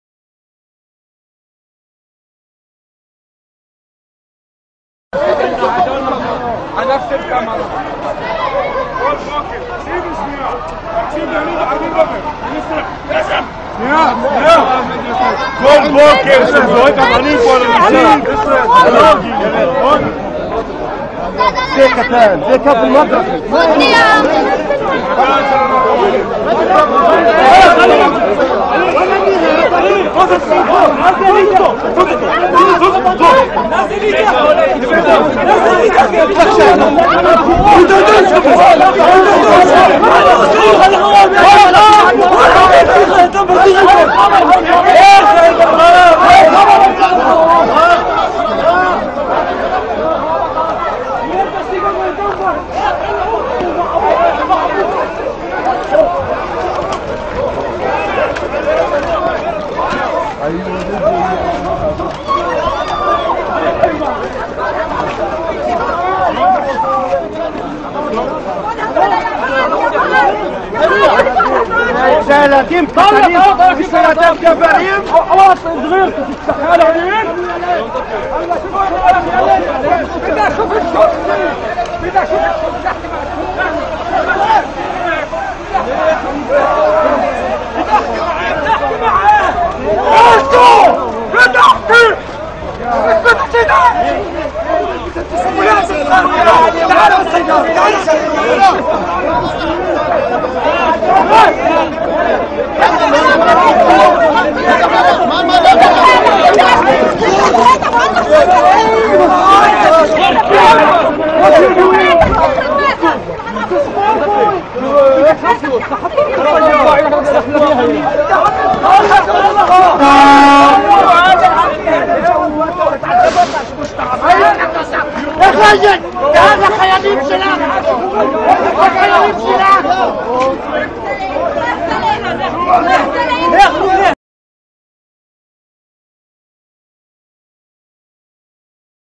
Des enfants palestiniens brutalisés par des soldats israéliens
Voilà le quotidien subi par les Palestiniens.